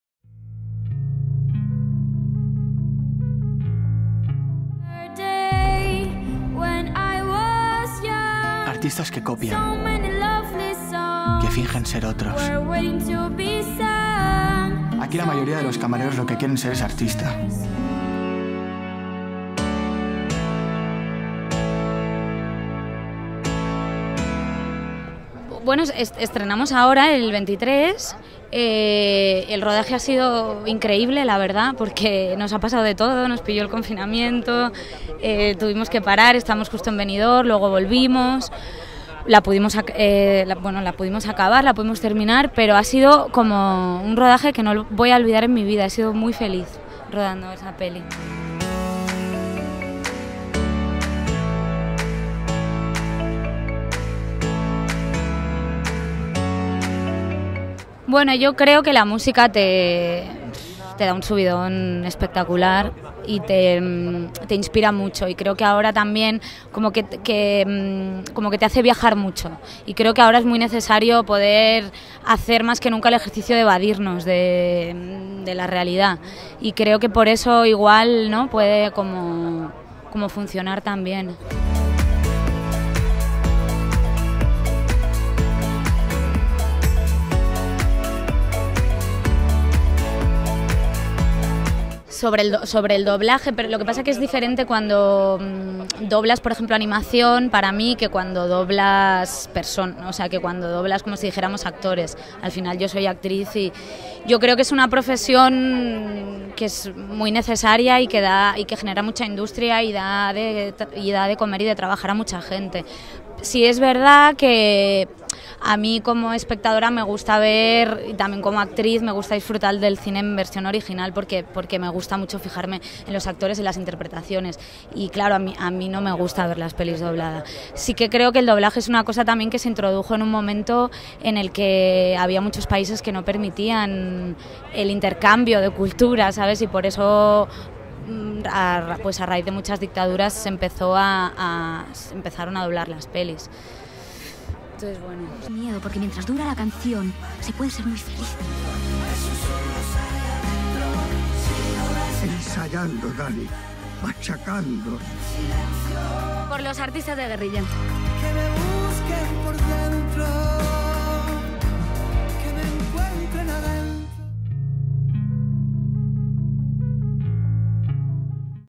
Marina Salas: "Como espectadora, me gusta disfrutar del cine en versión original"
En Cinemagavia hemos tenido la oportunidad de asistir a la presentación anual 2021 de la Revista Tapas. Entre los fulgurantes invitados, se encontraba Marina Salas. La actriz nos concedió una pequeña entrevista, donde nos habla de su experiencia en El Cover. Asimismo, nos expresa cuál ha sido su impresión de la recepción y del cariño con el que se ha hecho el film. A continuación, nos desvela su punto de vista sobre el auge del género musical en España. De esta manera, explica por qué cree que ha habido ese incremento en las producciones de este tipo.

Para terminar, tras la polémica con "Space Jam: Una nueva era" y el doblaje, nos confiesa qué es lo que opina de dicha profesión. Igualmente, nos comenta si le gustan las películas más en versión original, o, por el contrario, en su versión doblada. ¡No te pierdas la entrevista completa!